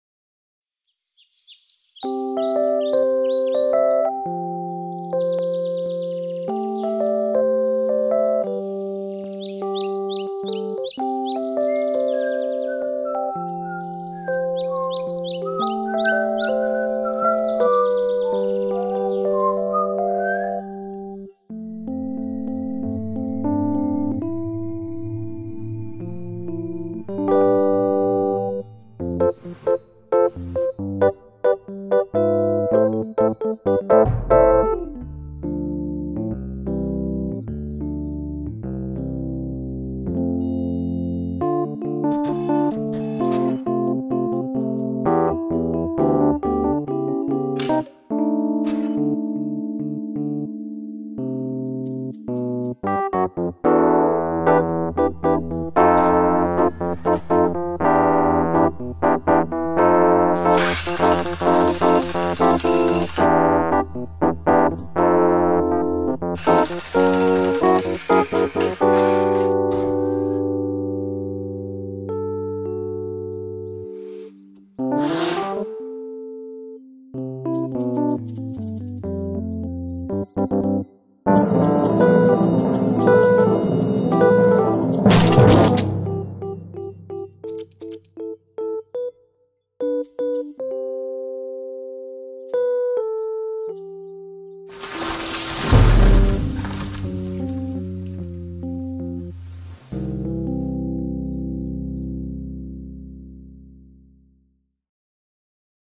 Choppy
Little Choppy went out for a walk in the woods to find a tree to chop for Chopsmas. Once he finds the perfect tree, Choppy also finds, that chopping -- is not that easy!